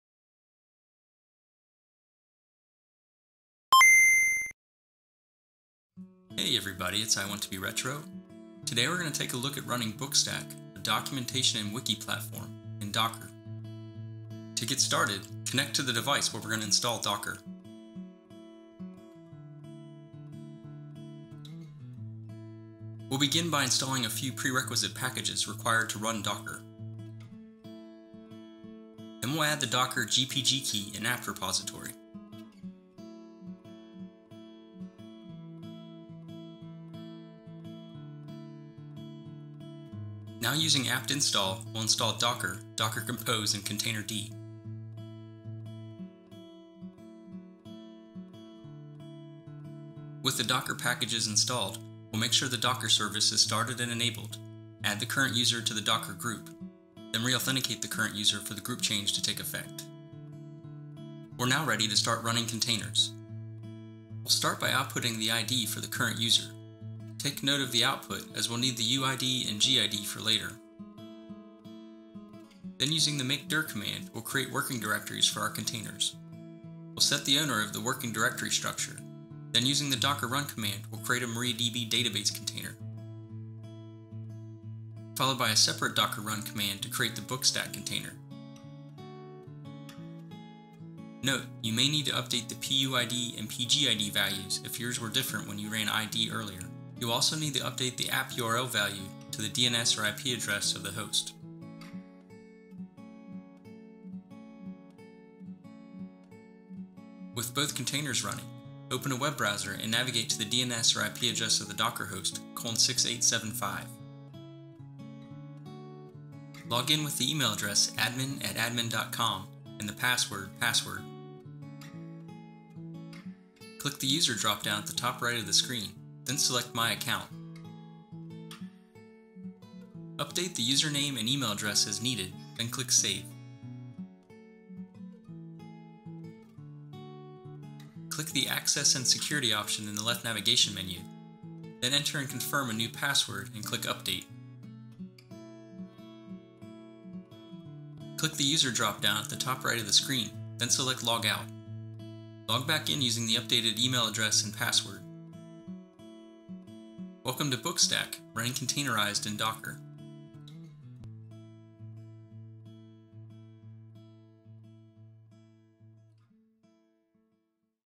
Run BookStack - Documentation/Wiki Platform - in Docker
What is BookStack?
BookStack is an opinionated wiki system that provides a pleasant and simple out-of-the-box experience. New users to an instance should find the experience intuitive and only basic word-processing skills should be required to get involved in creating content on BookStack. The platform should provide advanced power features to those that desire it but they should not interfere with the core simple user experience.

Installing Docker
   01. Log into the Linux based device
   02. Run the following commands in the terminal
         # install prerequisites
         sudo apt install apt-transport-https ca-certificates curl software-properties-common gnupg-agent -y
         # add docker gpg key
curl -fsSL -F'=' '/^ID=/{ print $NF }' /etc/os-release)/gpg | sudo apt-key add
         # add docker software repository
sudo add-apt-repository "deb [arch=$(dpkg --print-architecture)] -F'=' '/^ID=/{ print $NF }' /etc/os-release) $(lsb_release -cs) stable"
         # install docker
         sudo apt install docker-ce docker-compose containerd.io -y
         # enable and start docker service
         sudo systemctl enable docker && sudo systemctl start docker
         # add the current user to the docker group
         # reauthenticate for the new group membership to take effect
         su - $USER

Running BookStack
   01. Continue with the following commands in a terminal window
         # list current uid and gid, note these for later
         id $user
         # create working directories
         mkdir ~/docker/bookstack/config -p && mkdir ~/docker/mariadb -p
         # set ownership on the working directories
         sudo chown "$USER":"$USER" ~/docker -R
         # run the mariadb docker container
         docker run -d --name mariadb -e MYSQL_ROOT_PASSWORD=r00tp@$$ -e MYSQL_USER=bookstack_rw -e MYSQL_PASSWORD='B00k$t@ck!' -e MYSQL_DATABASE=bookstack -v ~/docker/mariadb:/var/lib/mysql -p 3306:3306 --restart=unless-stopped mariadb:latest
         # run the bookstack docker image
         # replace PUID, GUID with the output of the id $user command above
         # replace DB_HOST and APP_URL with the docker host DNS name or IP address
docker run -d --name bookstack -e PUID=1000 -e PGID=1000 -e TZ=America/New_York -e APP_URL= -e DB_HOST=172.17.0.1 -e DB_PORT=3306 -e DB_USER=bookstack_rw -e DB_PASS='B00k$t@ck!' -e DB_DATABASE=bookstack -p 6875:80 -v ~/docker/bookstack/config:/config --restart unless-stopped
02.
   04. Click the user dropdown at the top right ≫ My Account
   06. Select Access & Security from the left navigation menu
   07. Enter and confirm a new password ≫ Click Update
   08. Click the user dropdown at the top right ≫ Logout
   10. Welcome to BookStack